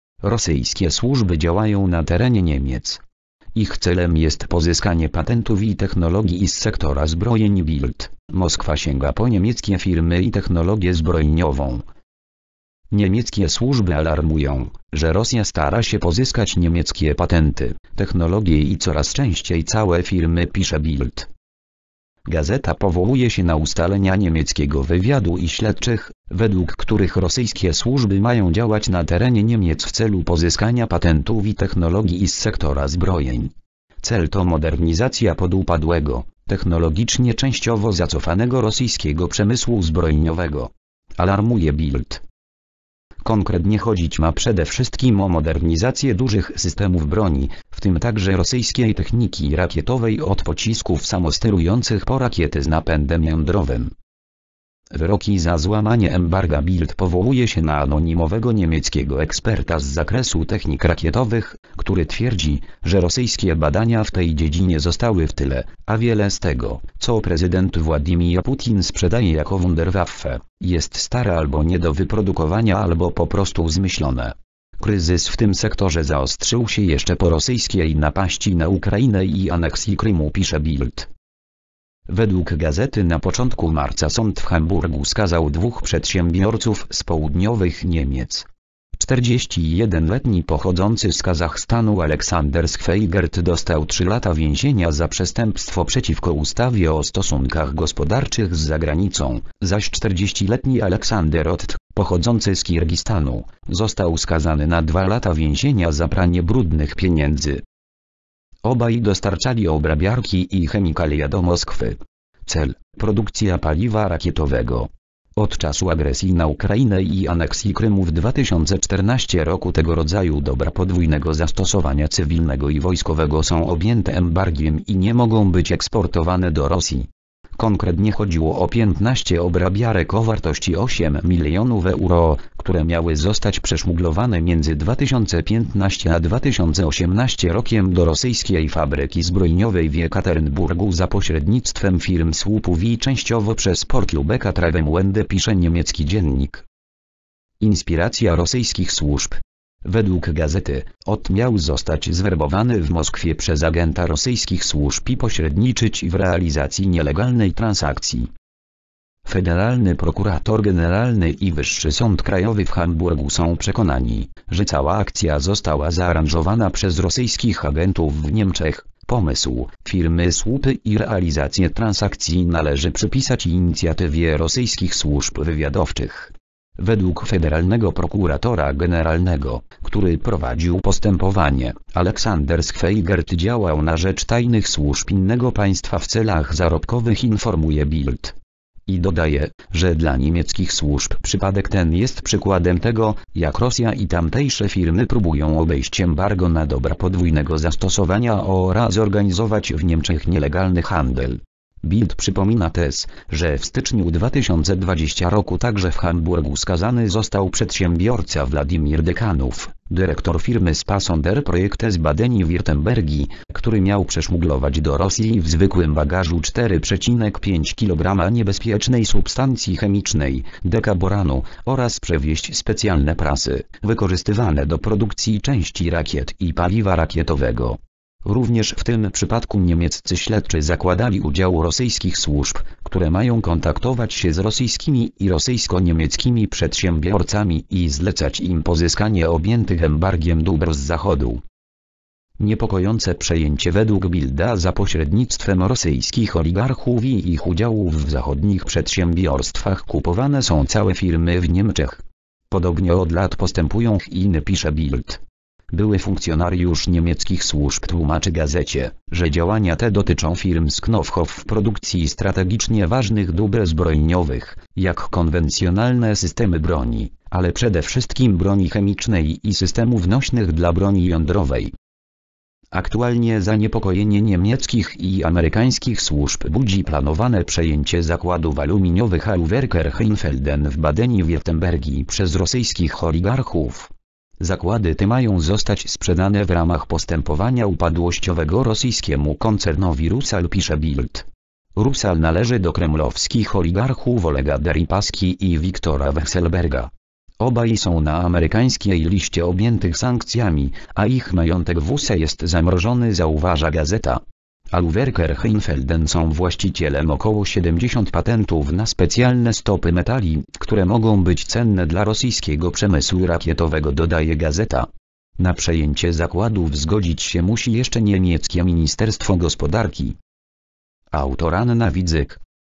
Rosyjskie służby działają na terenie Niemiec. Cel jest pozyskania patentów i technologii.
"Bild": Moskwa sięga po niemieckie firmy i technologię zbrojeniową.

   (fot. Alexander Koerner/Getty Images)